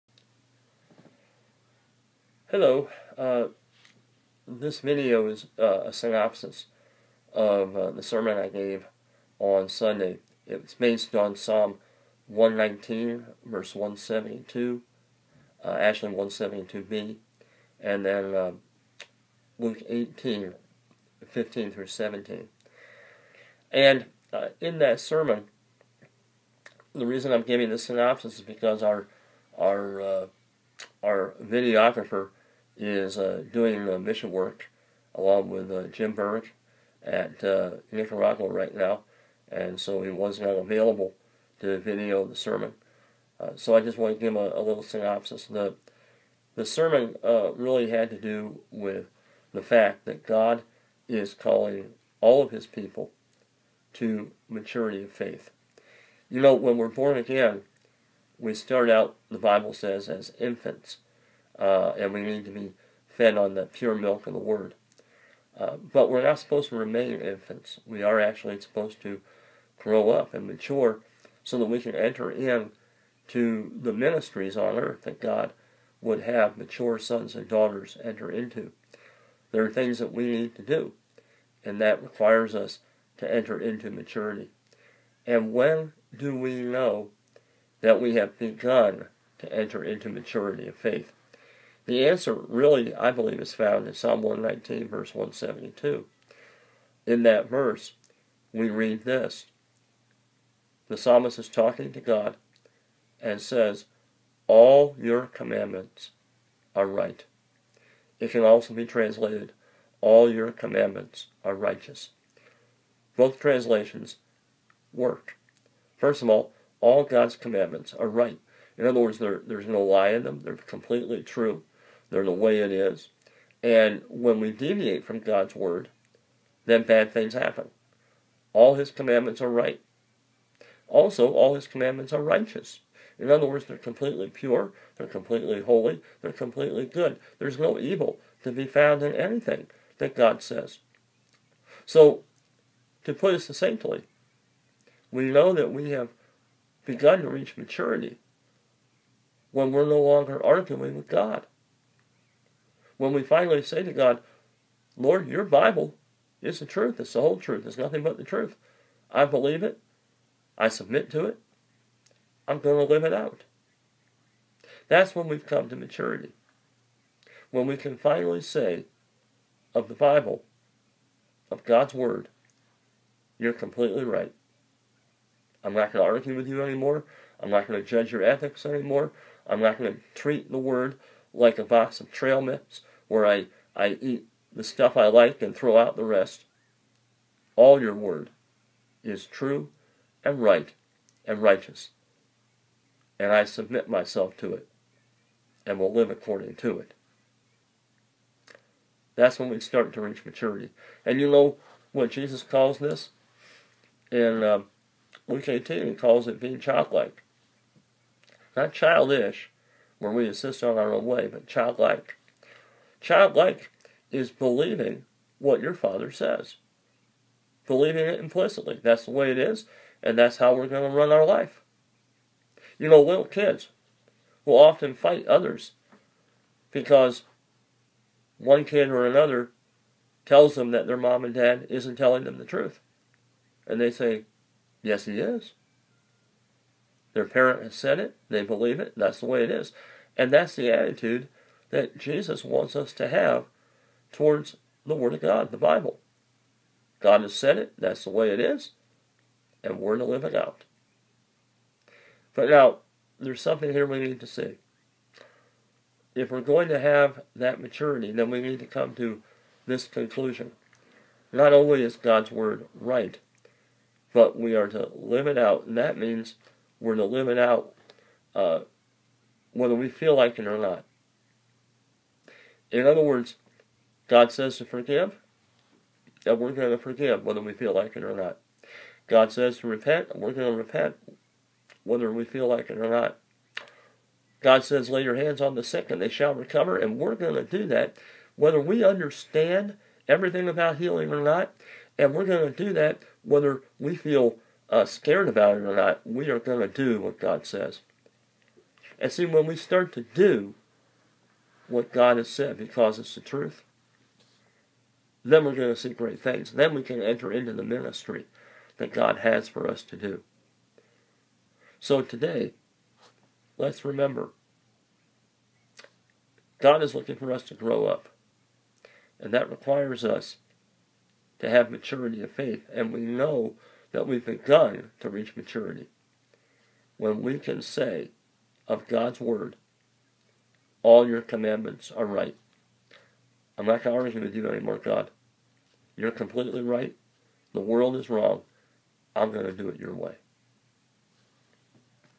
Synopsis of Sunday's Sermon Concerning Spiritual Maturity
Based on Psalm119:172b and Luke 18:15-17